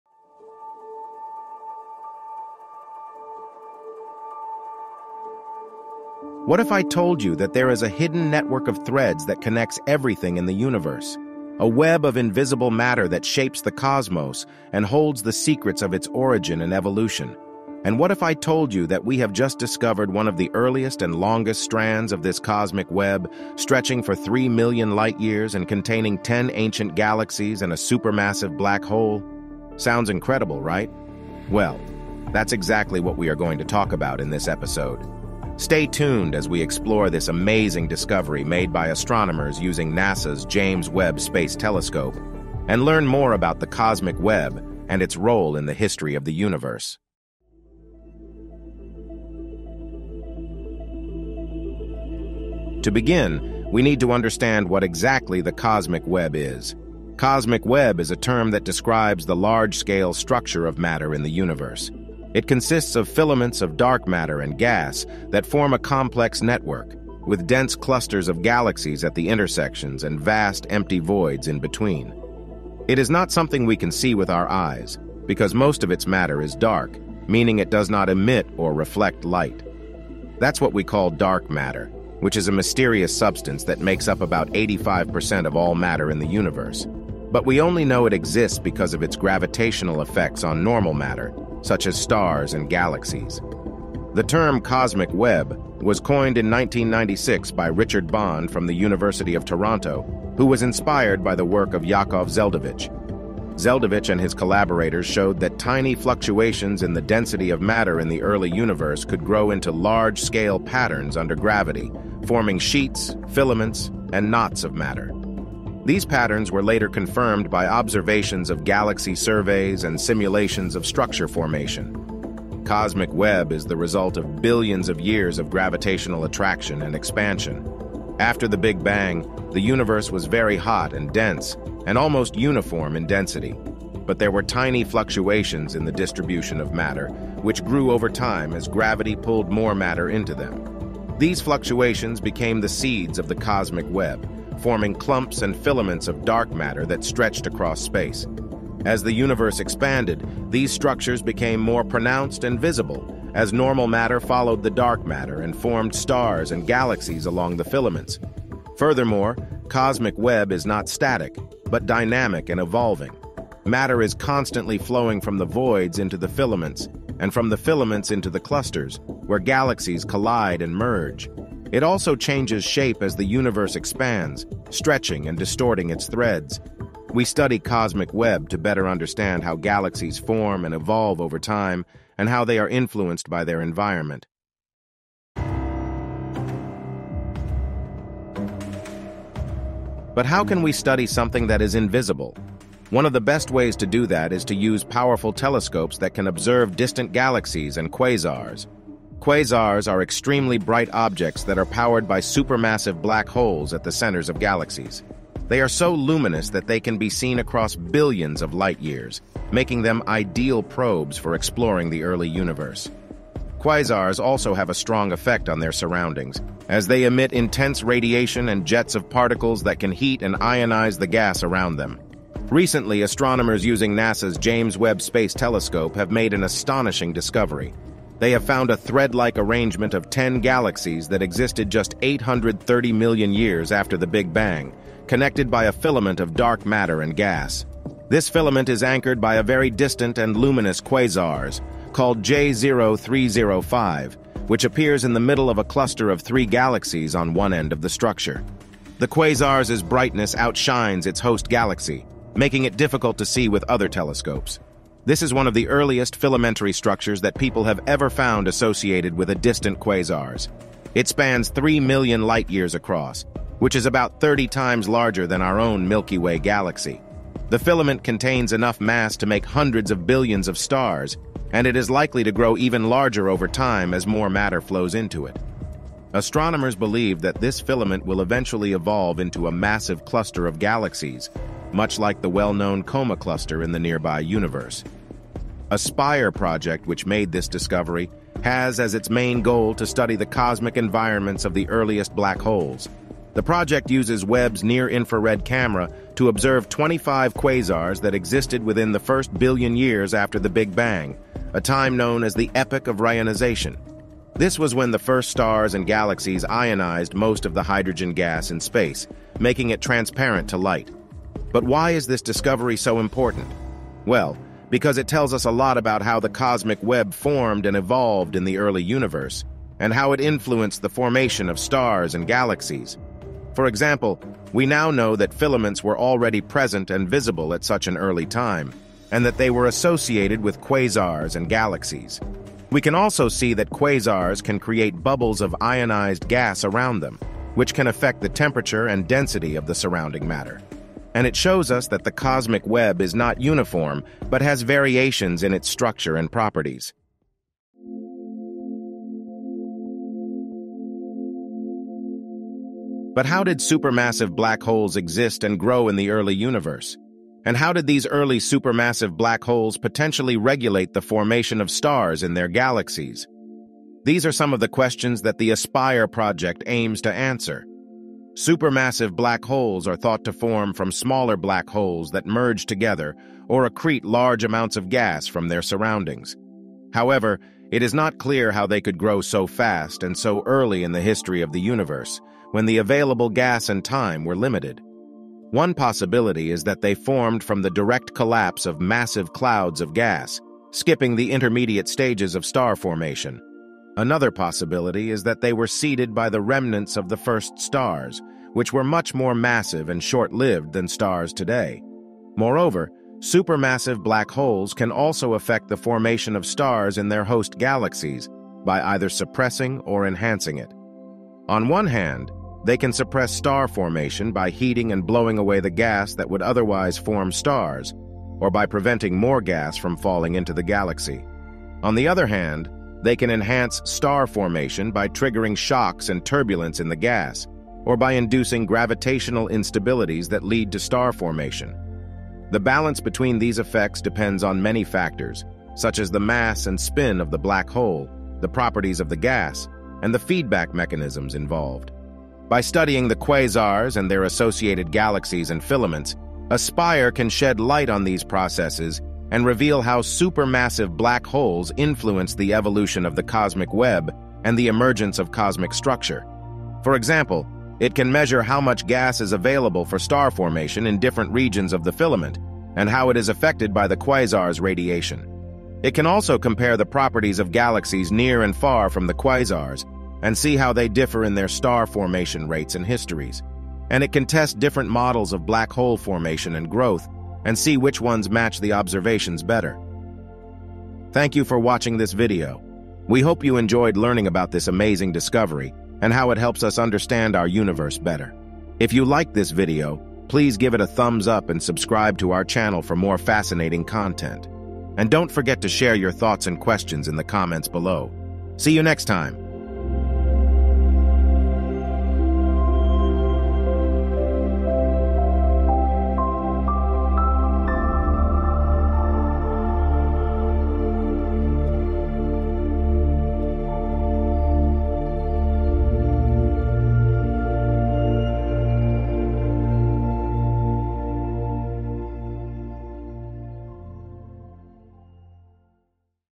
James Webb Detected a Mysterious Web of Dark Matter and Galaxies That Formed in the Early Universe!
Description: In this episode of Cosmic Web, we explore one of the most amazing discoveries made by NASA’s James Webb Space Telescope: a 3 million light-year long filament of dark matter and gas that contains 10 ancient galaxies and a supermassive black hole. This filament is one of the earliest and longest strands of the cosmic web, a network of dark matter and gas that connects everything in the universe. We will learn more about what the cosmic web is, how it formed and evolved, and how it influenced the formation of stars and galaxies. We will also learn how supermassive black holes played a crucial role in shaping the cosmic web and its structure. Join us as we uncover the secrets of the cosmic web and its hidden network of dark matter and galaxies.